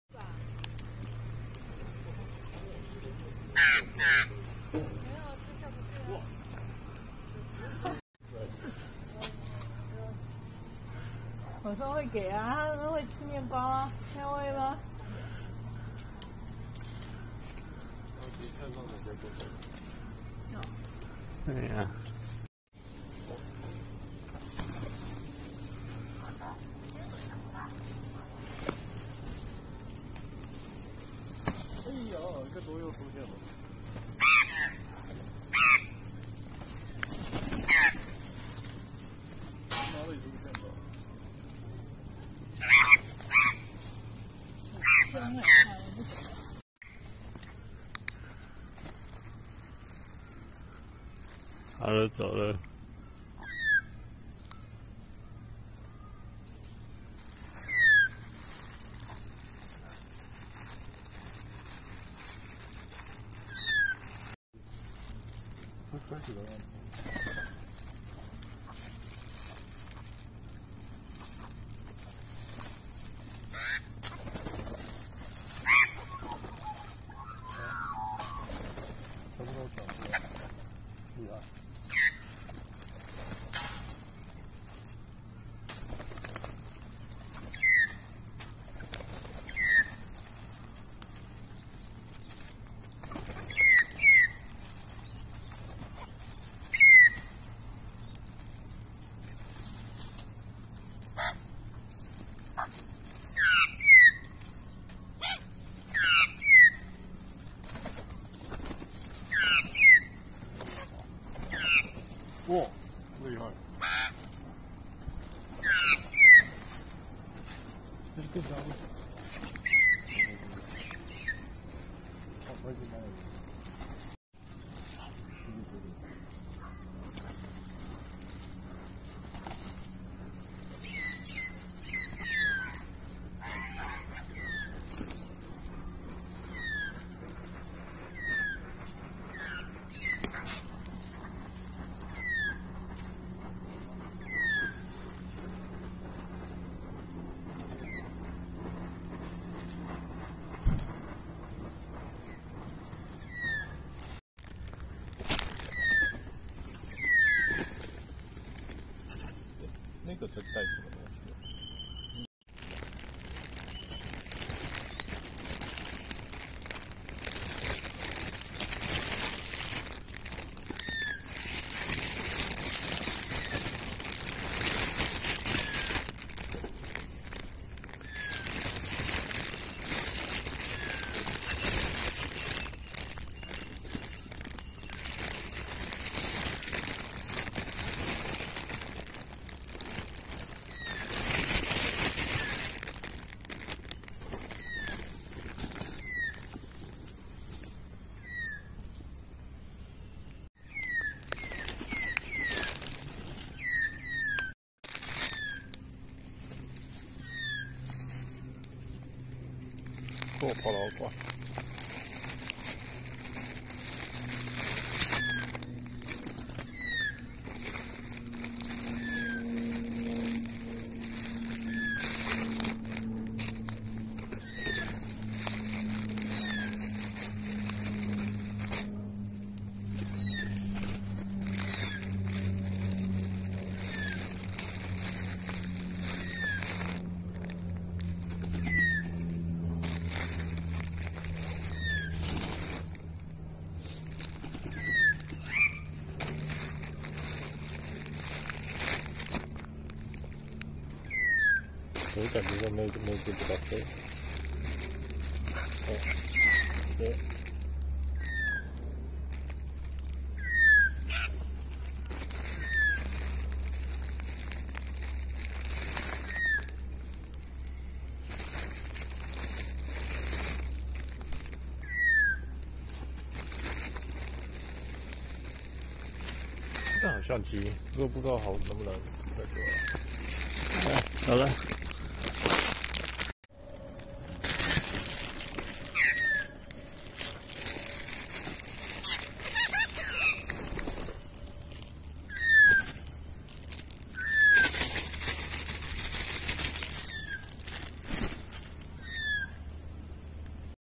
澳洲土火雞與喜鵲搶麵包 Alectura lathamiCracticus tibicen at Hawkesbury River full
澳洲土火雞，學名：Alectura lathami. 英文又稱 Australian Brush-Turkey。實際上跟火雞沒有很近的親緣關係。

叢塚雉又名澳洲叢塚雉、大塚雉，是一種常見的、分布廣泛的塚雉，其分布於從遠北昆士蘭到新南威爾斯州南海岸的尤若波達拉。澳洲叢冢雉也被引入南澳大利亞的袋鼠島。它是冢雉科現存最大的物種，是唯三棲息於澳大利亞的塚雉之一

澳洲喜鵲（學名：Cracticus tibicen）是澳大利亞極為常見的鳥類， 平時待人親和，但一到繁殖季節，地域性就變得特別強烈。鳥巢附近方圓 100 到 150 公尺內的自行車騎士，是最頻繁的攻擊目標； 其次為行人；再來則是跑者。